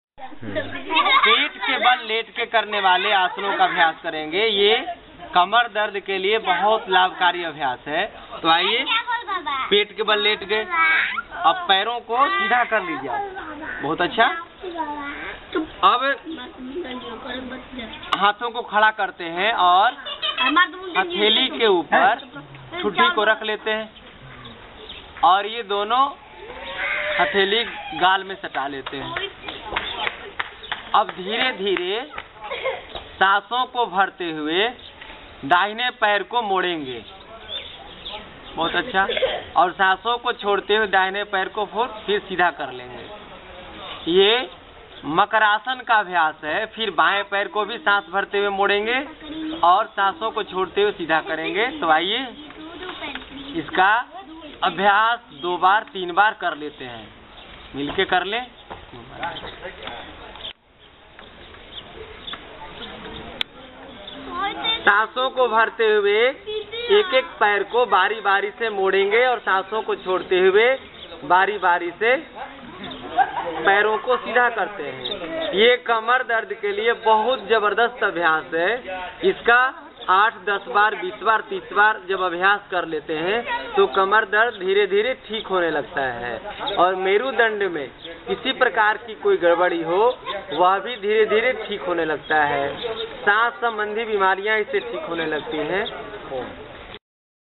Yog Shivir Bargaon|Brij Mohan |Patanjali Yogpeeth-Haridwar|Agiaon,Bhojpur,Bihar|28-11-2016
DSCN1184
05-State-BIHAR,District-BHOJPUR,Month-November-2016,Camp No.-05,Time-5.30AM TO 8.30AM,Yog Shivir-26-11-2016 To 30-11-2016,Vill-Bargaon,Post-Bargaon,Block-Agiaon,District-Ara(Bhojpur),State-Bihar,Video Cliping  Date-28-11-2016